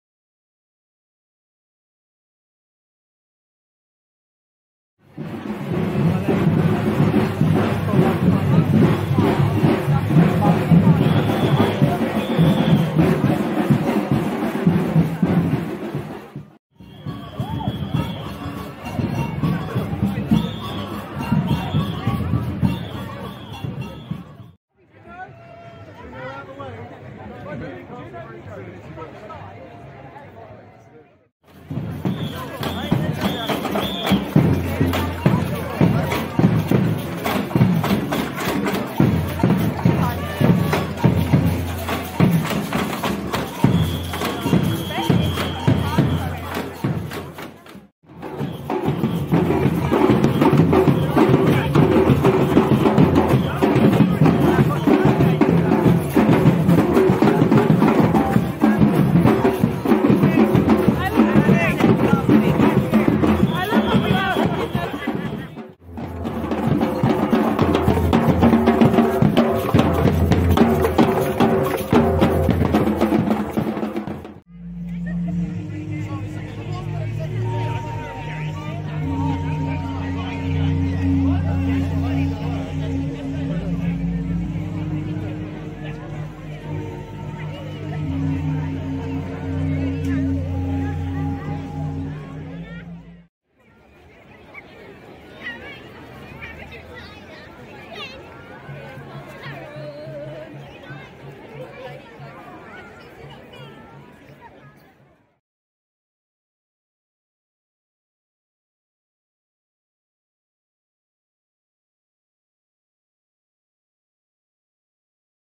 Burning the Clocks Parade - 2022
Burning the Clock is an event that takes place in Brighton, UK on 21/12 each year to mark the shortest day of the year. There is a parade of lanterns followed by a bonfire where the lanterns are burnt.